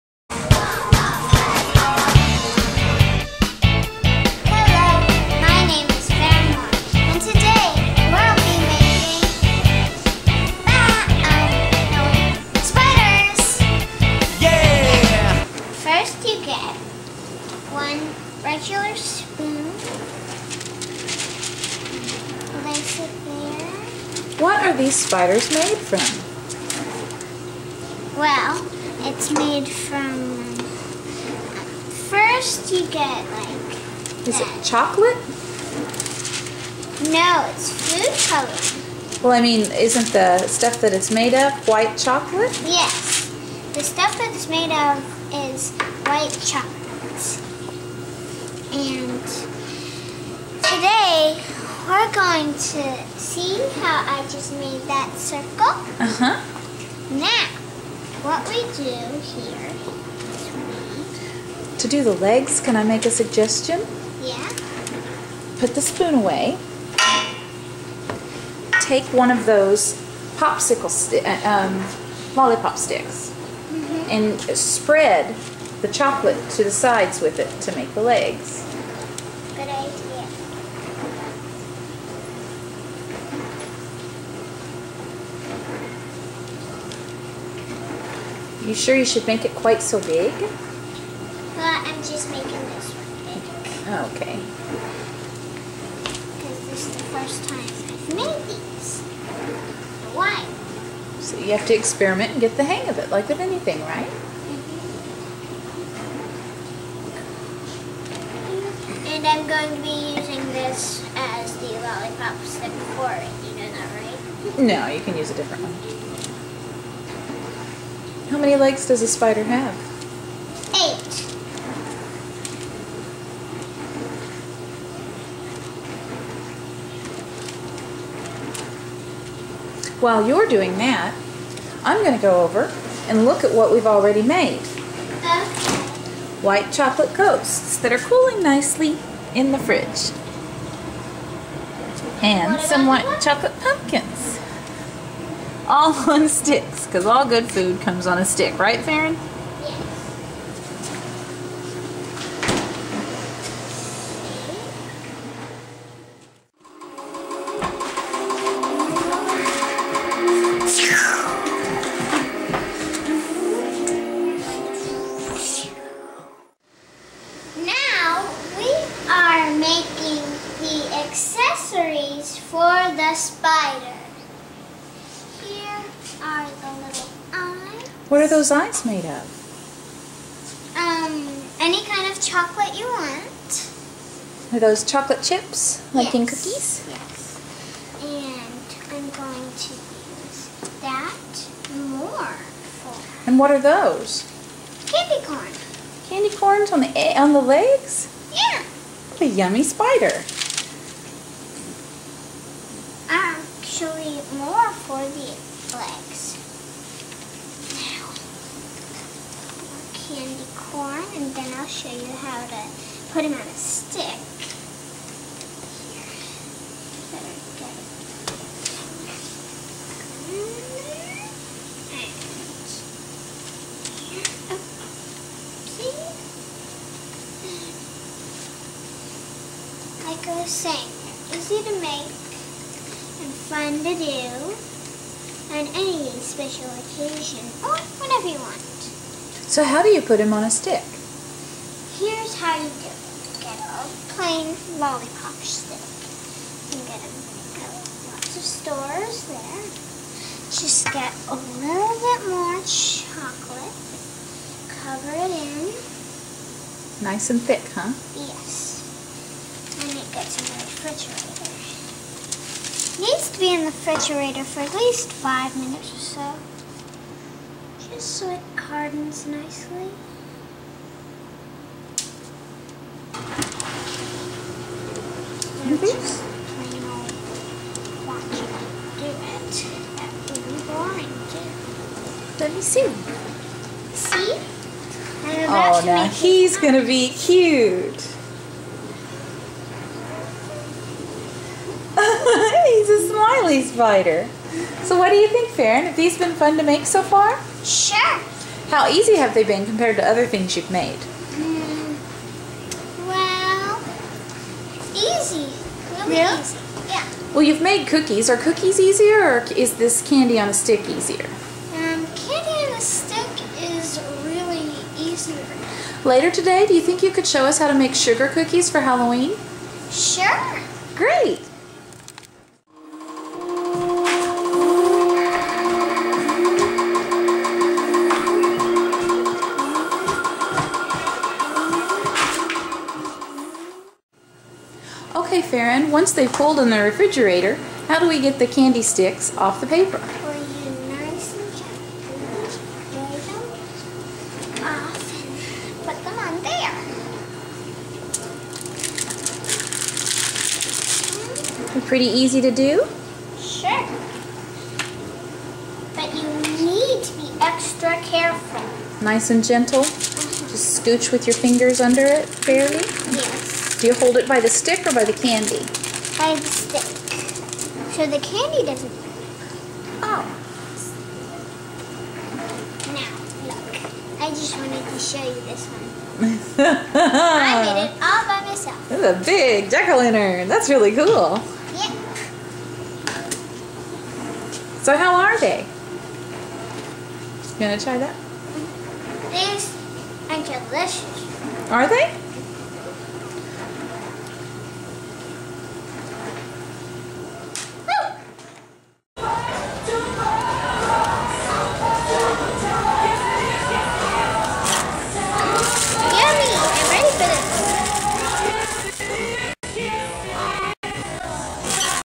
Great Adventures with Faeron Mor - Episode 1
Chocolate Spiders!

Melt white chocolate chips and add food color to make just about anything fun on a stick.  It takes only 5 minutes in the fridge for them to cool and harden.